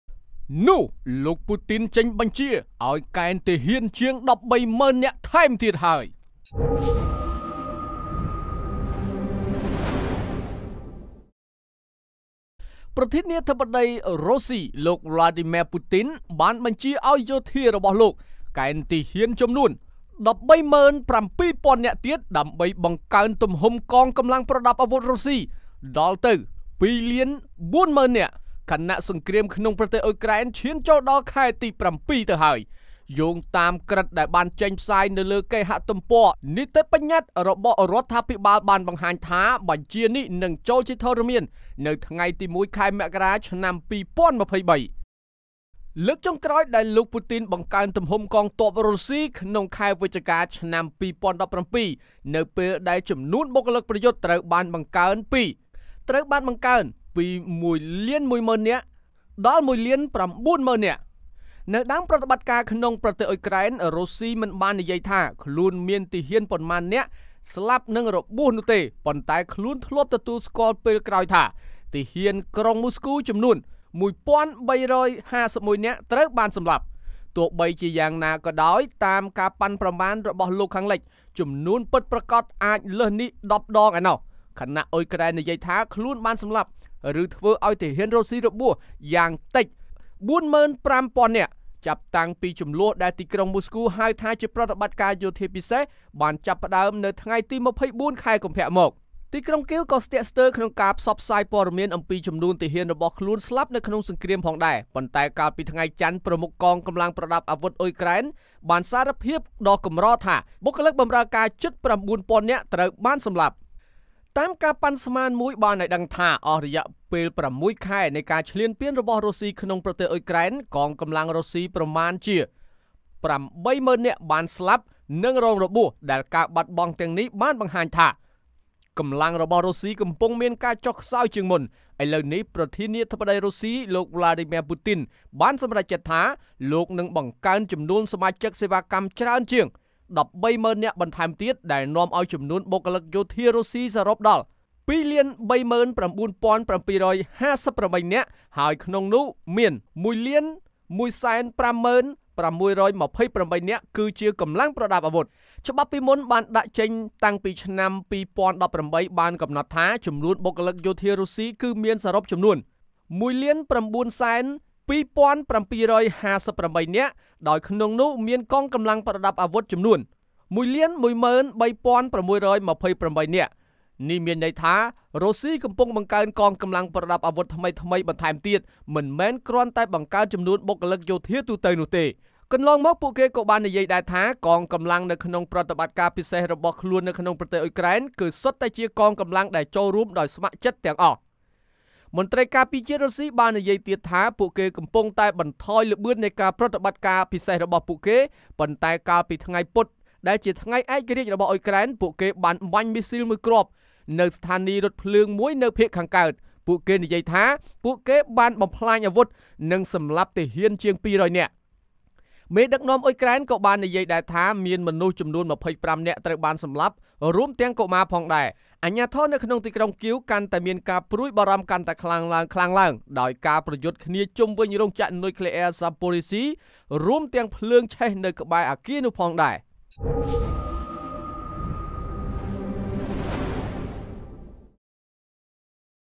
នោះរុស្សីលេងខ្នាតធំហើយហើយ......ពូទីន ចេញបញ្ជាឲ្យ កែនទាហាន ជាង ១៣ម៉ឺន នាក់ទៀត
សូមអភ័យទោសរាល់កំហុសឆ្គង
Tell: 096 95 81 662